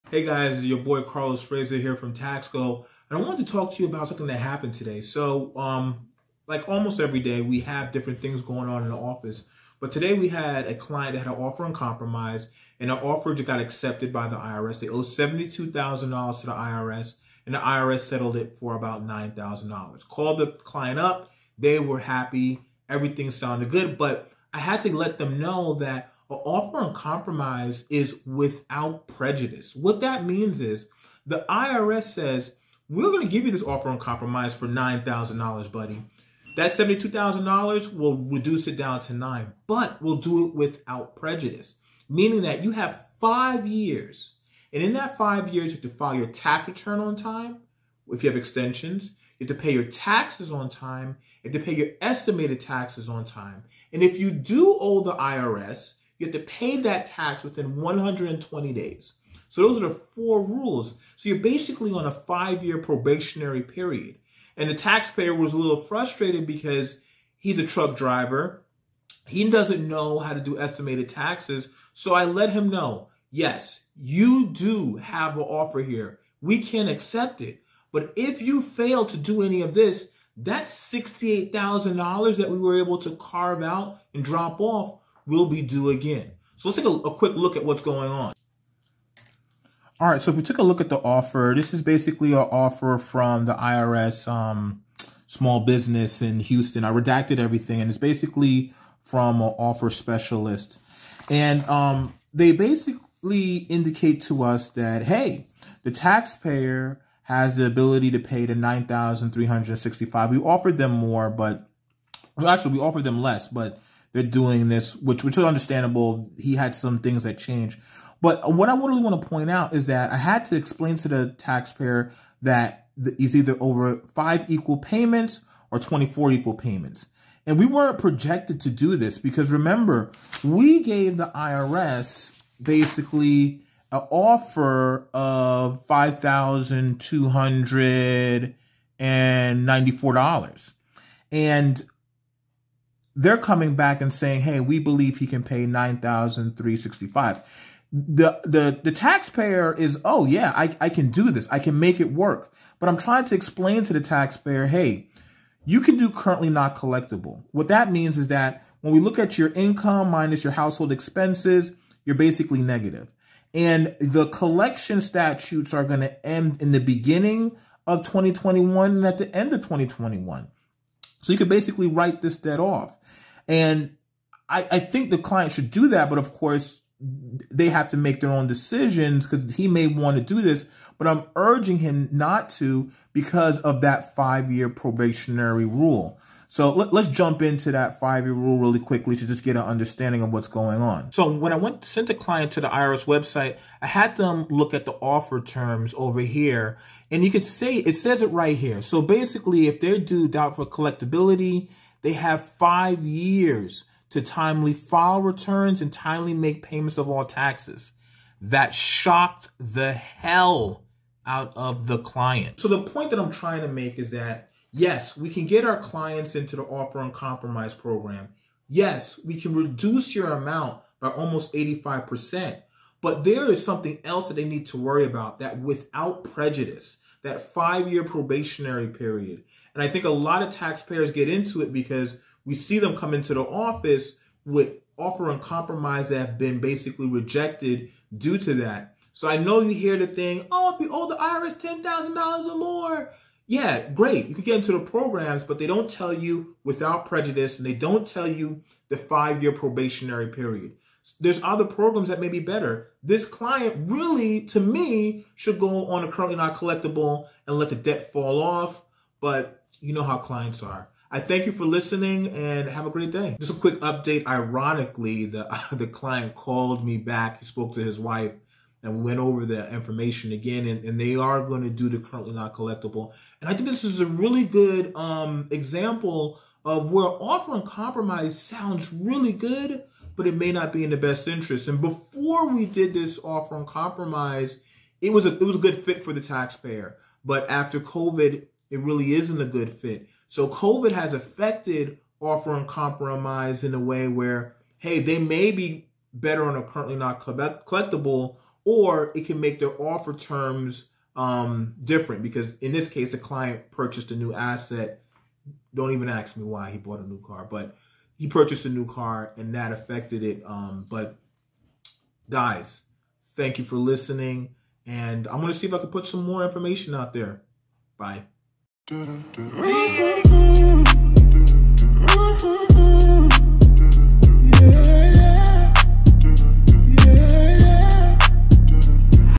IRS REDUCE TAXES BY $60K BUT WE SAID NO! SEE WHY
This is a good video about how time can change an IRS Tax reduction program plan. With this case the following happened:
• We filed the offer with the IRS in November of 2019 but it was accepted on December of 2020.
• In 2019 due to the fact the tax debts were coming to an end in less than 12 months; they would have been forced to go on a payment plan for $5,833 a month. Or $1,700 a month based on the disposal income.
• Covid-19 happened and we amended the offer, the taxpayer’s income went down. (Yes, you can amend an Offer in Compromise from 656)
• The taxpayers purchased a new asset that had some equity. That was the only reason for the increase in the offer.
• Due to the 12 months it took to get accepted with the IRS and the debt will expire. Plus, the fact that Covid-19 took a toll on the taxpayer we decided to not accepted the offer and went into the IRS program to not pay for 12 months. This would let the debt fall off and no money out the taxpayer’s pocket. ( Due to the fact it took so long to resolve the life of the loan lived a little longer)

This is a great example of looking at all the options and how the tax reduction plan can transform like bumblebee.  That will prequalify you for ALL IRS programs in 10 minutes or less.

IRS Terms:
1. You have to file your taxes on time for the next 5 years.
2. You have to pay all your estimated payments in the next 5 years on time.
3. If you owe the IRS in the next 5 years you have to pay the amount off in full.
4. If you cannot pay in full you have in 120day to pay in full (Maybe 180 with COvid-19 not sure)